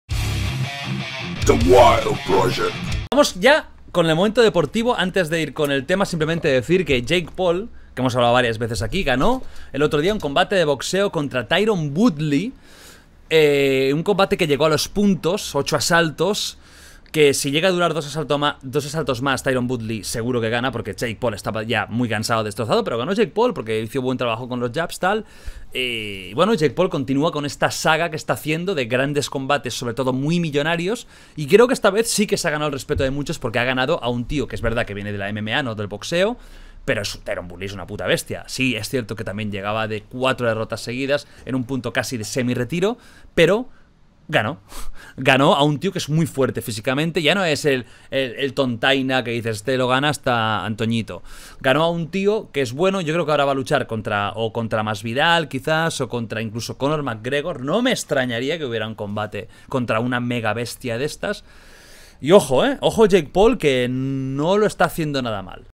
JAKE PAUL GANA SU COMBATE CONTRA LA BESTIA TYRON WOODLEY. Y YA.
¡Vuelve The Wild Project después de un descanso de verano! En la tertulia de esta semana, las noticias más impactantes y curiosas de los últimos días: la restricción total de los videojuegos en China, el hombre que se selló con pegamento el miembr0, el ex-marine británico que decidió salvar antes a animales que a personas en Afganistán, la crisis absoluta medioambiental en Ciudad de México, el triste fallecimiento de Tomii11, y mucho más. ¡No te lo pierdas!